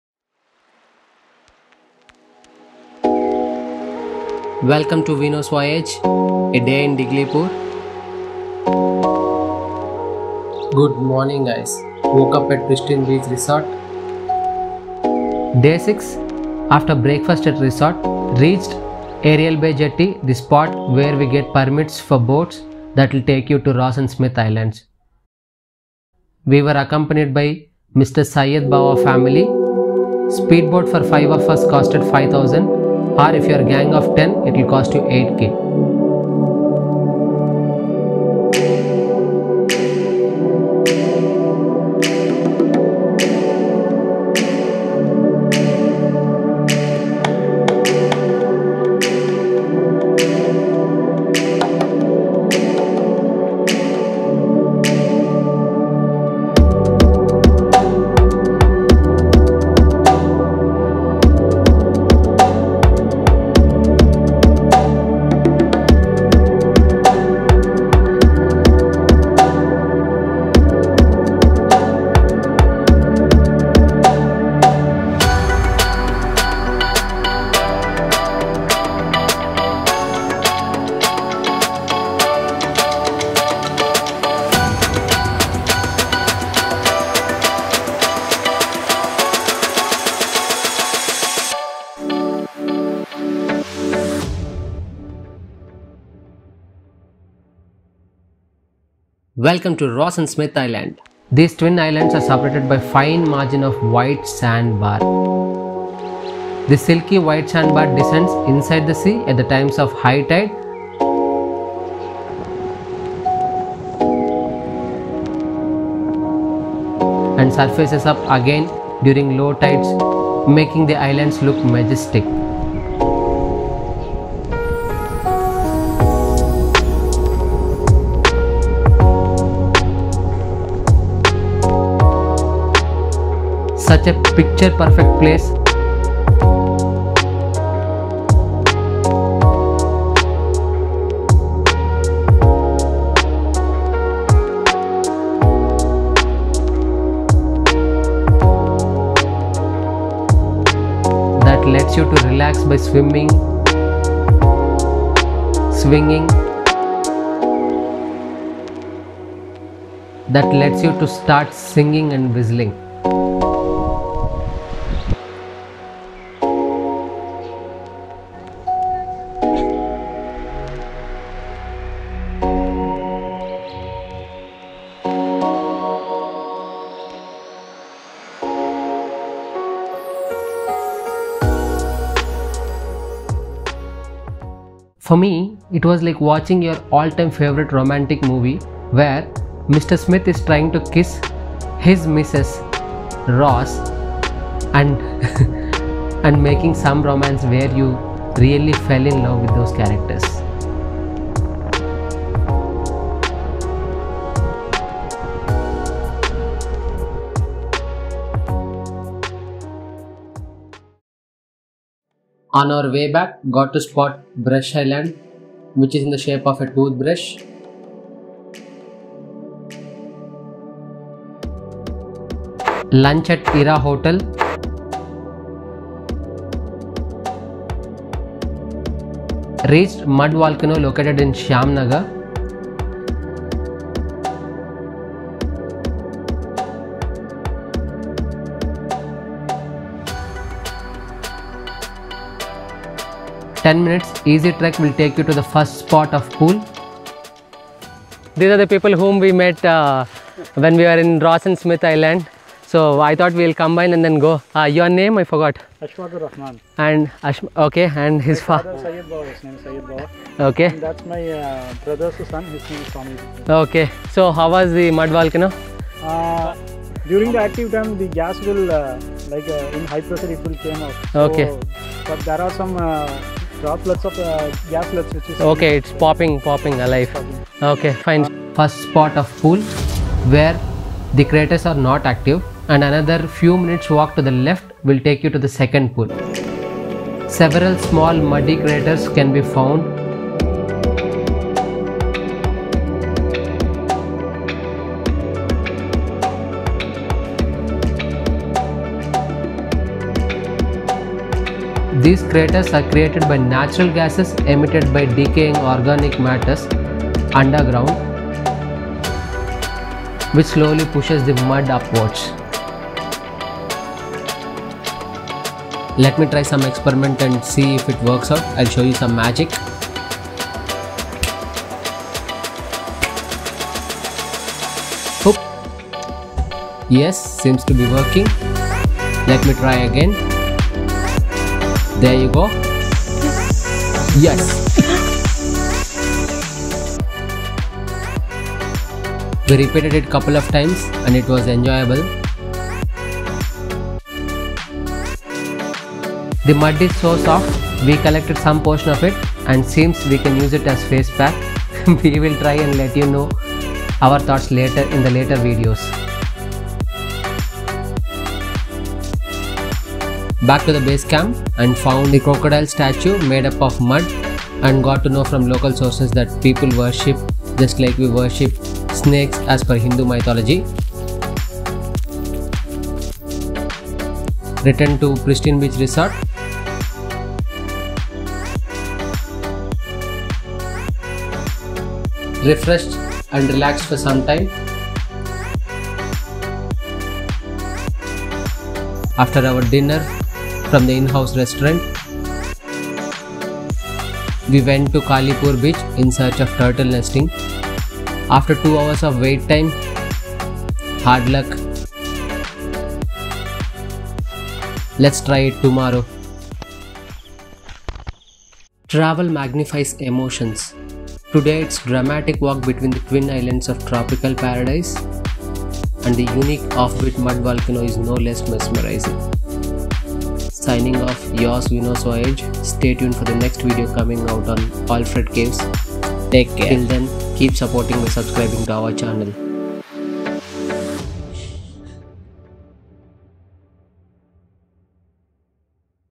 Ross & Smith Island | Mud Volcano | Day6 | Ep-6 | Diglipur | North Andaman | VinoS Voyage
Mesmerising walk between Twin Islands Ross and Smith, The Fire experiment on Mud Volcano was just indescribable. we found this on day2 of North journey which is our Day6 of Andaman.

Need to reach Ross and smith Island:
- Reach Aerial Bay jetty
- get permits by submitting your Aadhaar original.
- You can Hire each boat by 5000/- if you wish else
- wait to team of 5 with coming tourists and pay 5000/- and leave for the island else
- wait to team 10 with coming tourists and pay 8000/- and leave for the island.

Mud Volcano:
No permits required, reach Shyam nagar, need to walk for 10 mins and its a evident path to reach.enjoy a little time and come back.

Mandatory to travel to Middle and North Andaman:
- Cowin double vaccination certificate or Negative RAT is mandatory.
- Adhaar origin.
- Permission to travel through Jarawas from Andaman govt.

Stay in Diglipur:
1. pristine beach resort:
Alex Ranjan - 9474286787
2. Turtle Resort(Govt.  stay)

Helpful Tips:
-  Breezy clothes.
-  offline maps
- carry some eatables bcoz early find some shops.
- novels if u are habit of reading
- offline songs/ videos/ games
- get along with driver to know about locals
- find spotting tribals when u travel from north to south or vice versa.
- Enjoy flora, fauna via forest.
- Sunscreen
- Shades.
- Languages spoken Bengali/Hindi/Tamil/English/Telugu.
- Weather condition can change any time of the day, so have a spare day to accommodate your plans & not disturb your entire itinerary.